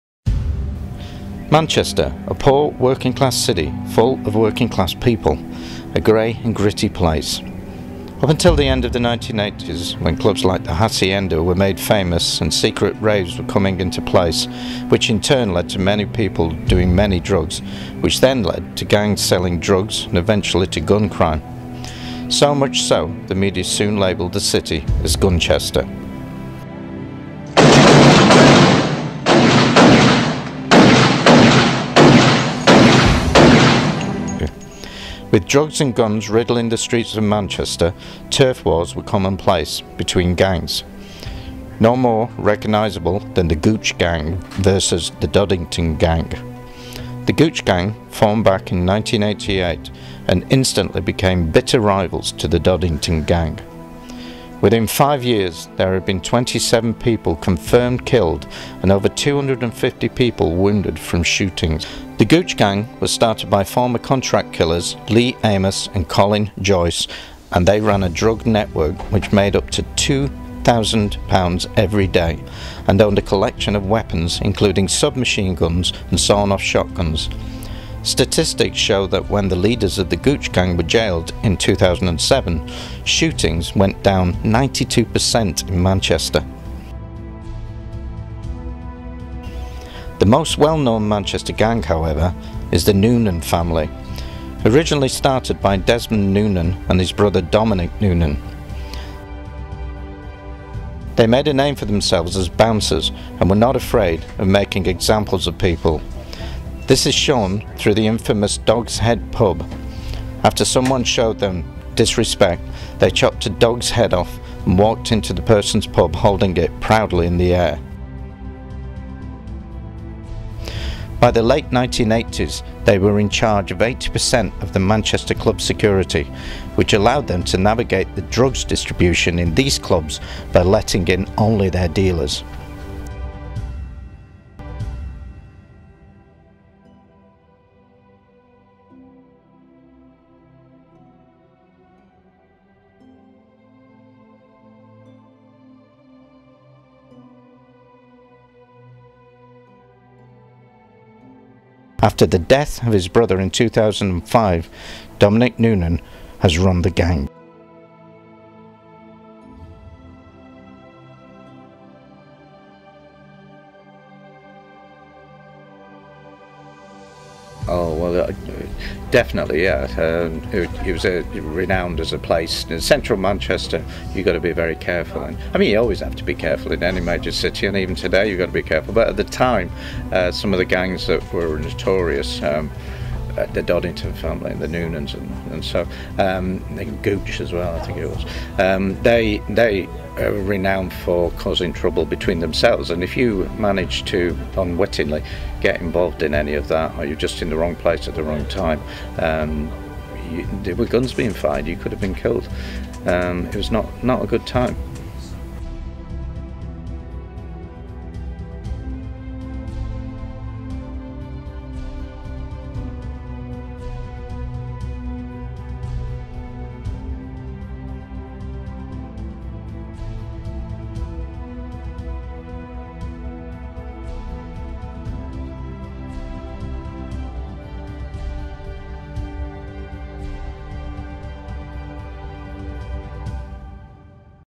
Stockport College - 2014 - 2016 - Documentary - Ben
As part of the Extended Diploma in Creative Media: Television and Film course at Stockport College, students are required to research and make a 5 minute documentary on a historical event or figure. This project is designed to teach the students the importance of research and how to produce an informative and factually accurate programme whilst at the same time making it entertaining to watch.